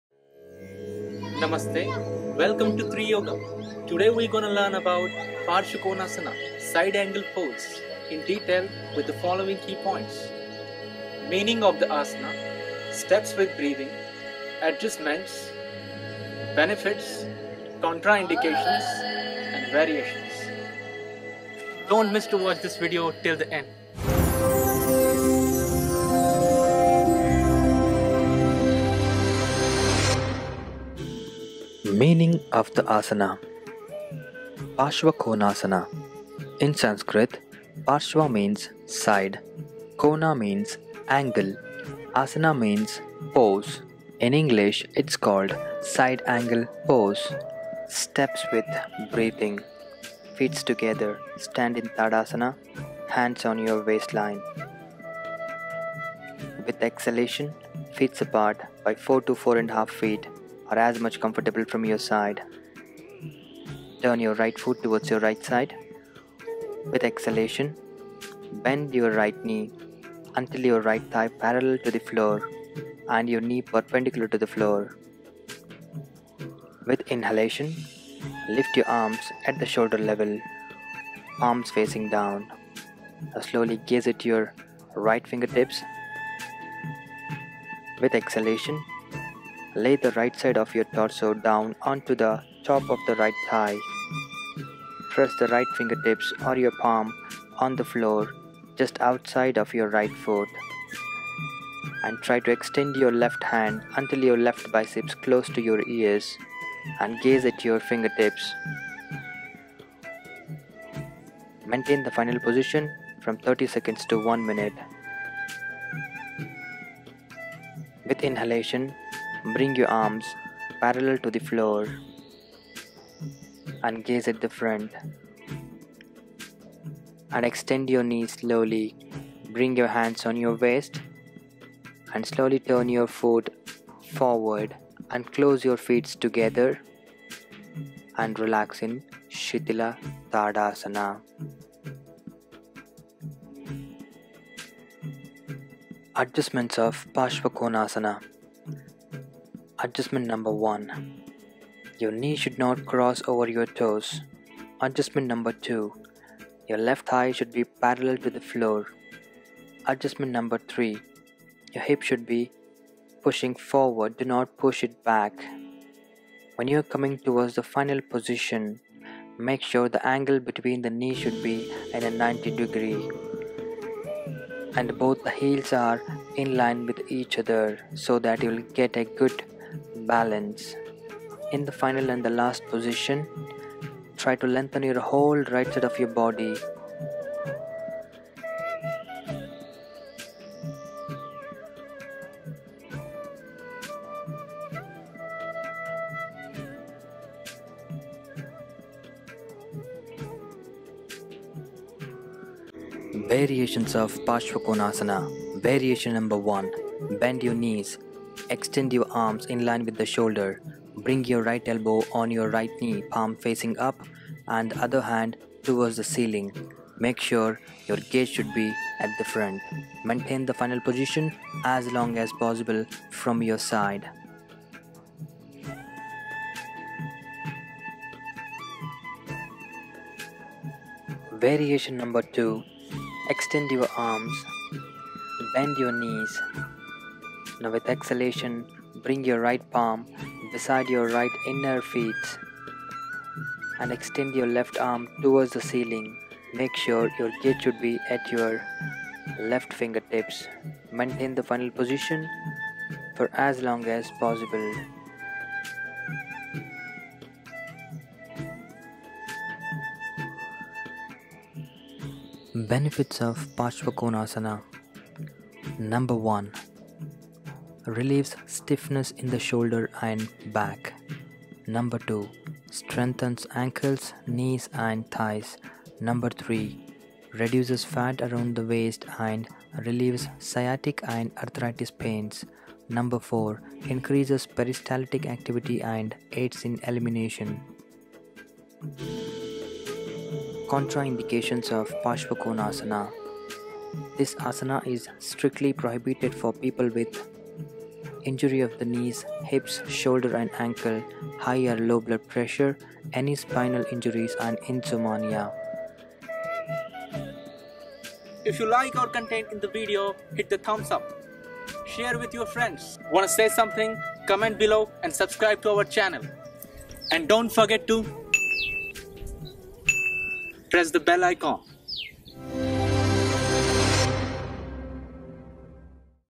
Side Angle Pose (Parsvakonasana): Yoga for beginners | Triyogam Series
Introducing Side Angle Pose (Parsvakonasana)  also called as Extended side angle pose as a part of Yoga For Beginners series from Triyogam with the following details:

1. Meaning of Side Angle Pose (Parsvakonasana)
2. Side Angle Pose (Parsvakonasana) Steps with Breathing
3. Side Angle Pose (Parsvakonasana) Adjustments
4. Side Angle Pose (Parsvakonasana))Benefits
5. Side Angle Pose (Parsvakonasana) Variations
6. Contraindication to Side Angle Pose (Parsvakonasana)
Hope this Video would have covered almost all the details about the asana,
Hey there! Please Visit Yoga Videos for beginners

Yoga Instructor: Suresh Desai
DOP & Edited by: KR Bharat Kumar
Concept: Gaurav Kumar
Music Composer: Shakeel Ahmed.
Recording & Mixing by: Dream Studio